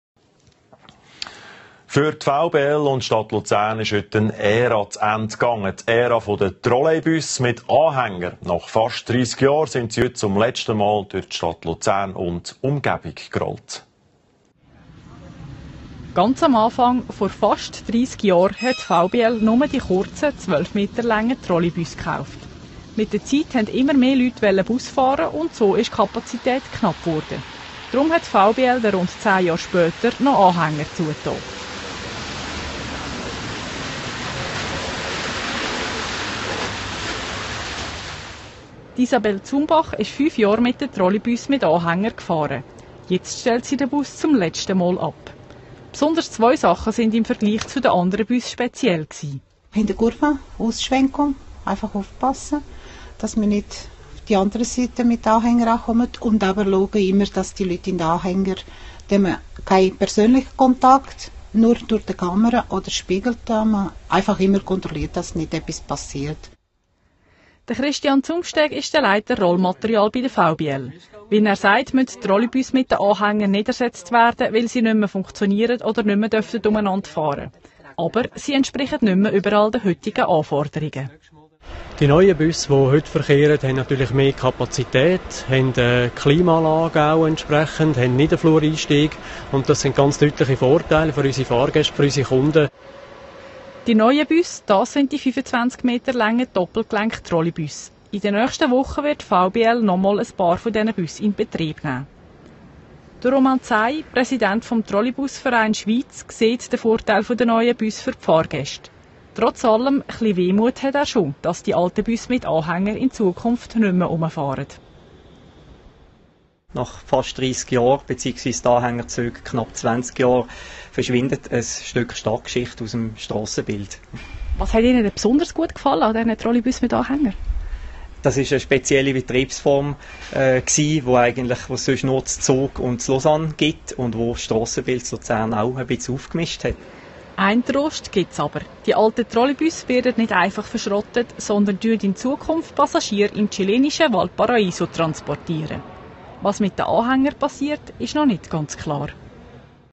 Reportage des VBL à propos des NAW de Lucerne 🚎
Le reportage de la Télé de Lucerne a tenu à rendre hommage aux Trolleybus NAW des VBL.
Lors de la dernière course d'adieux.
Nouvelle vies bientôt pour Valparaìson en Chili 🇨🇱.